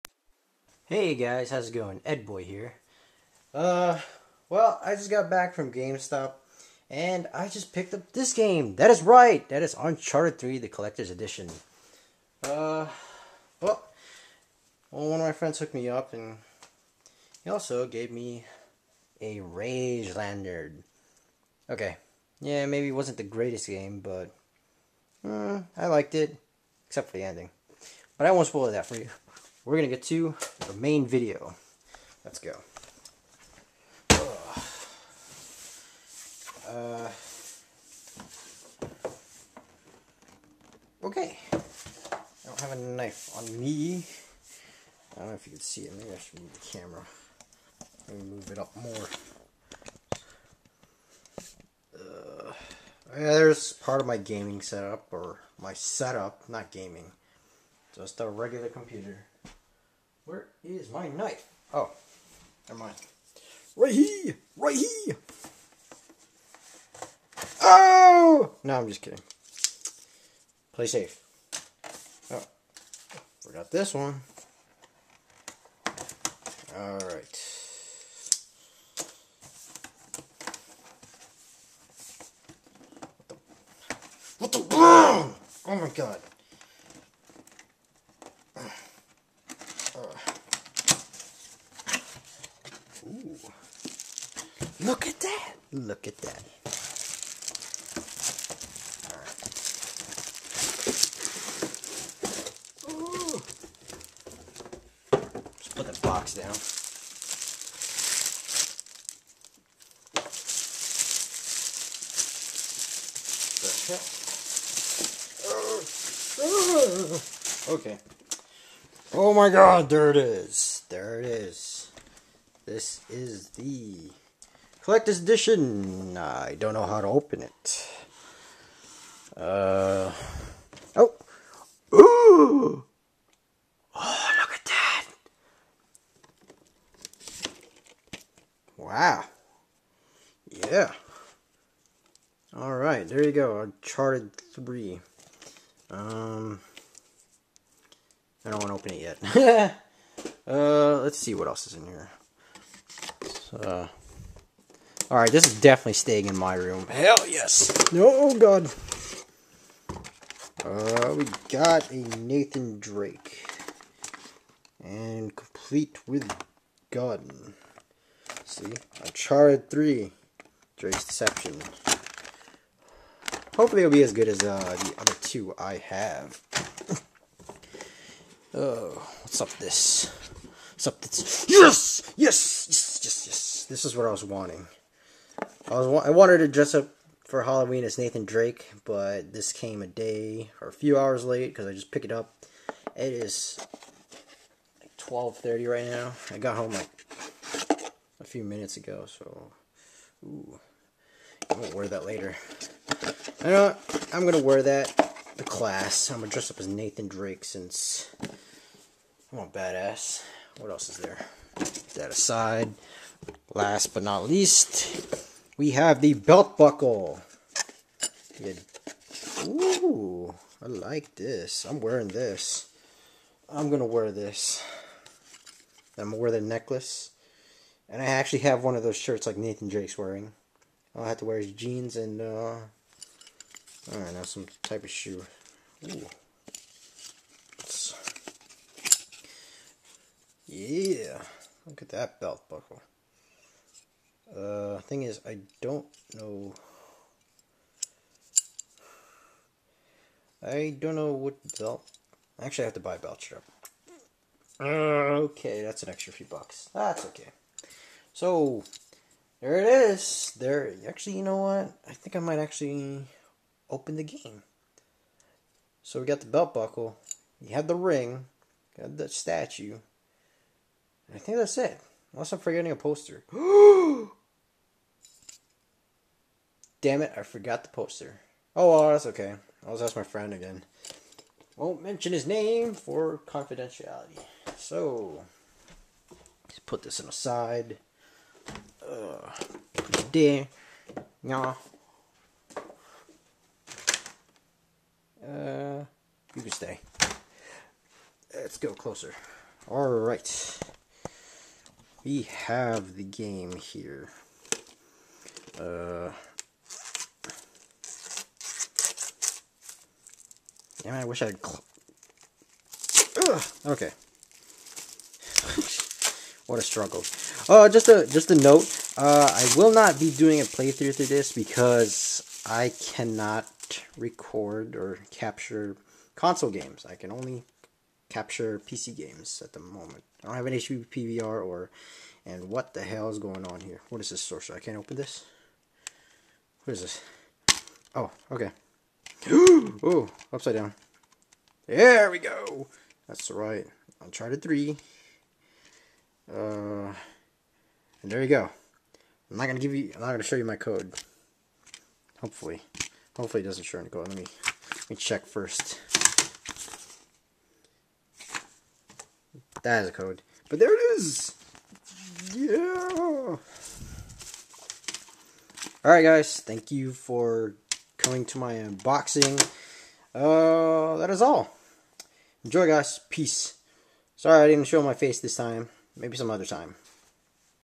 Uncharted 3 Drake's Deception Collectors Edition Unboxing
My first unboxing video with one of my favorite game series Uncharted.